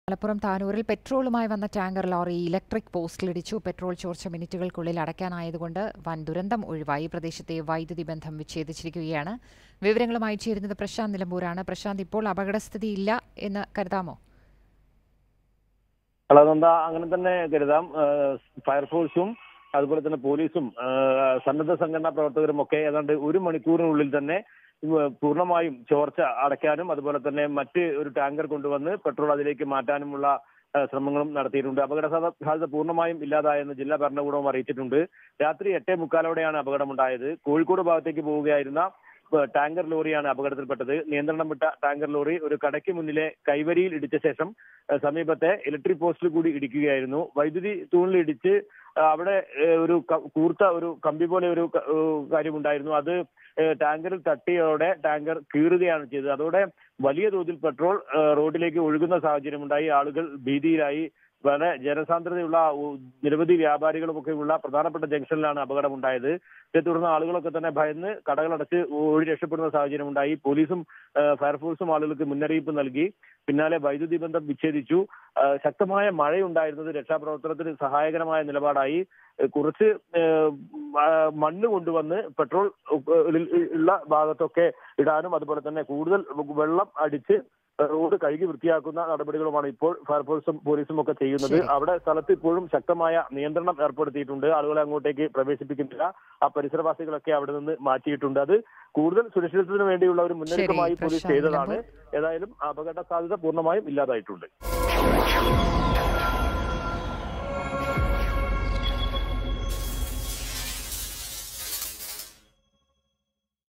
പെട്രോളുമായിവന്ന ടാങ്കർ ലോറി അപകടത്തിൽപ്പെട്ടു Petrol tanker lorry accident
മലപ്പുറം താനൂരിൽ പെട്രോളുമായിവന്ന ടാങ്കർ ലോറി വൈദ്യുതി പോസ്റ്റിലിടിച്ചു, പെട്രോൾ ചോർച്ച മിനിറ്റുകൾക്കുള്ളിൽ അടയ്ക്കാനായതുകൊണ്ട് ഒഴിവായത് വൻ ദുരന്തം
petrol tanker lorry hits electric post at malappuram tanur